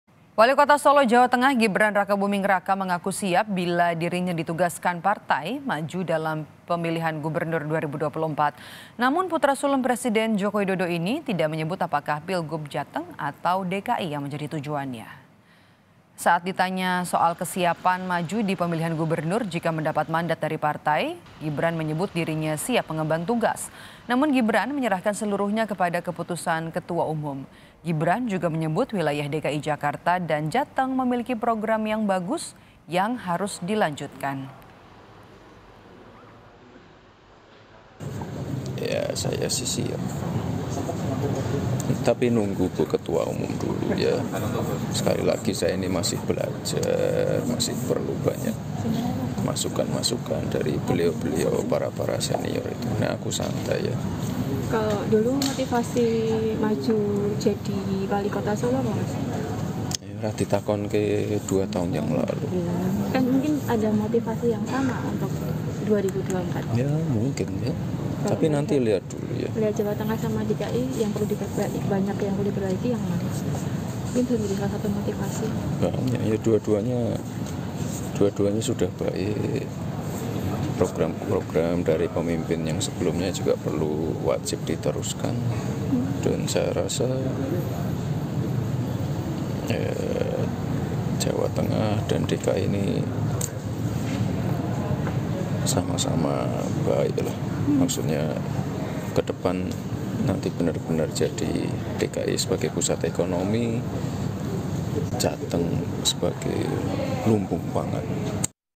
Ditanya Soal Maju Pilgub 2024, Gibran Mengaku Siap Jika Dapat Mandat Megawati
SOLO, KOMPAS.TV - Wali Kota Solo, Jawa Tengah, Gibran Rakabuming Raka mengaku siap apabila ditugaskan partainya maju dalam Pilgub 2024.

Namun putra sulung Presiden Joko Widodo ini, tidak menyebut apakah Pilgub Jateng atau DKI Jakarta tujuannya.

Saat ditanya soal apakah siap maju di pemilihan Gubernur jika mendapat mandat dari partai, menyebut dirinya siap mengemban tugas.

Namun Gibran menyerahkan seluruhnya pada keputusan ketua umum.

Gibran juga menyebut, wilayah DKI Jakarta dan Jateng memiliki program bagus yang harus dilanjutkan.